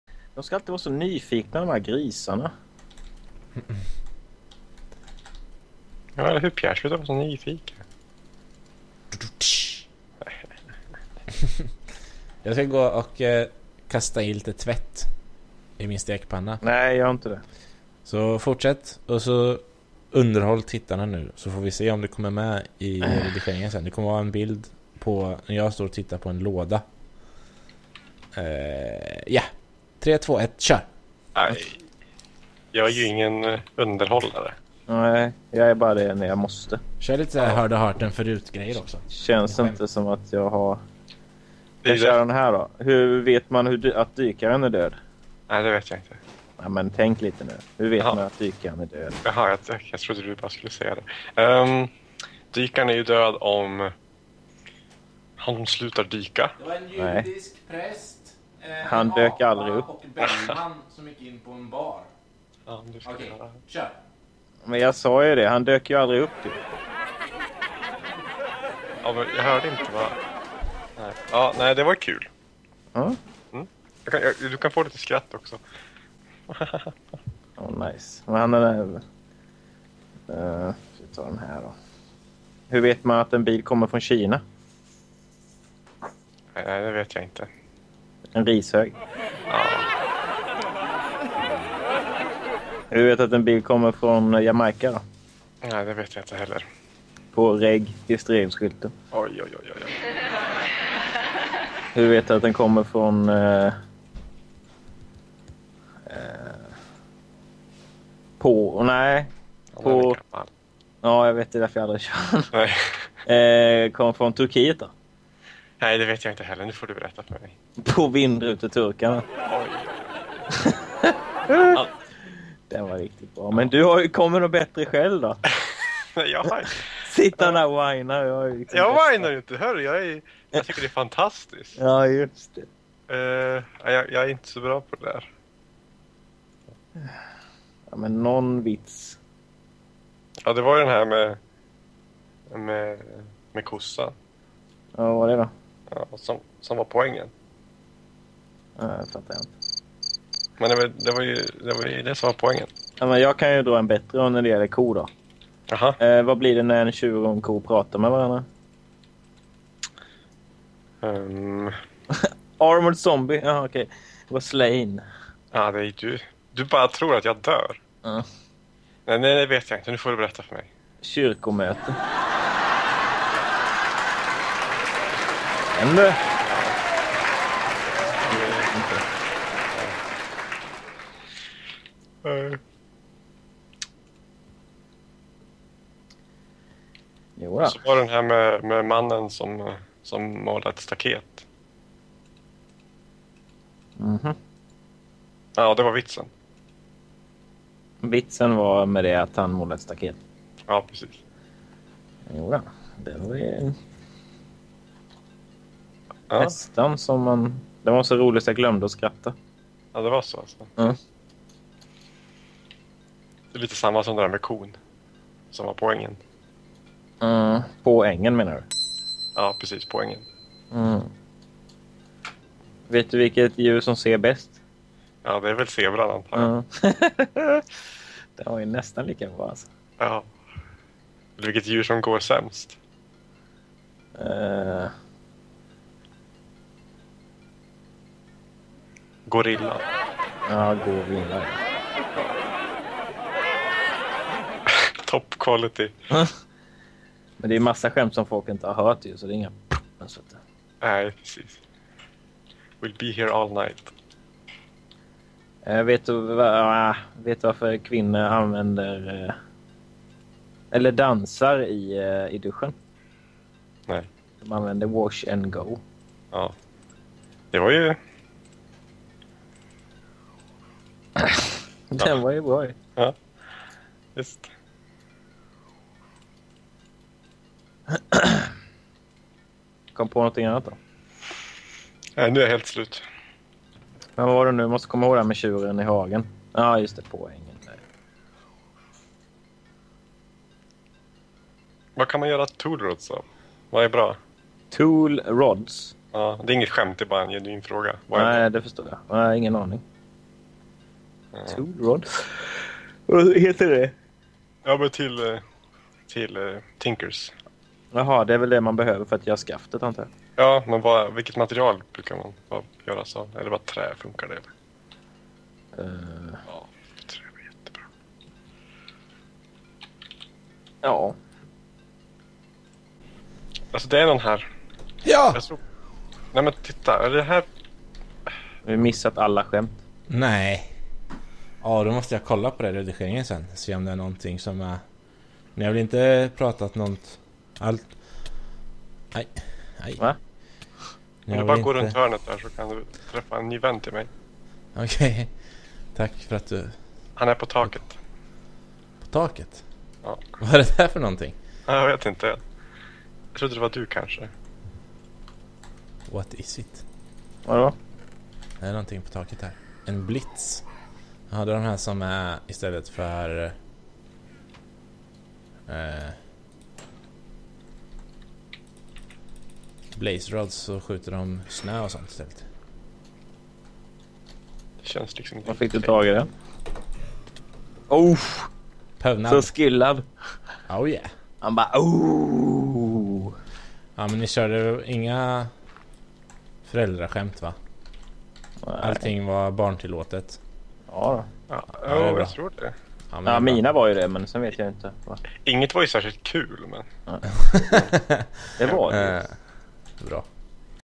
Josstick Enterprice Del 2 - #9 - Extra material [OBS! Torra skämt, alltså sjukt torra]
Här kommer lite skämt som blev bort klippt i avsnitt 9...

♥ Tyckte du om denna video? Gilla den! Det uppskattas verkligen.